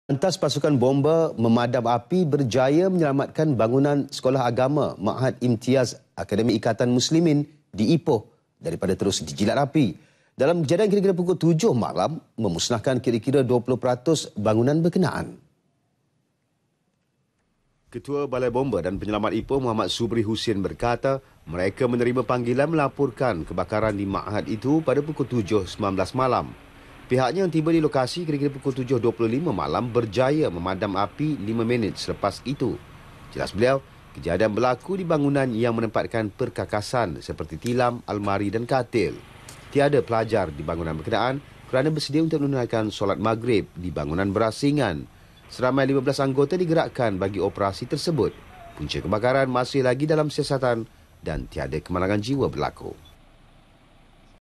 KEBAKARAN MAAHAD TAHFIZ IMTIZAZ – TINDAKAN PANTAS BOMBA ELAK KERUGIAN BESAR [28 MAC 2018]
Tindakan pantas pasukan bomba memadam api berjaya menyelamatkan bangunan sekolah agama Maahad Imtiyaz Akademi Ikatan Muslimim di Ipoh daripada terus dijilat api.
Dalam kejadian kira-kira pukul 7 malam , memusnahkan kira-kira 20 peratus bangunan berkenaan.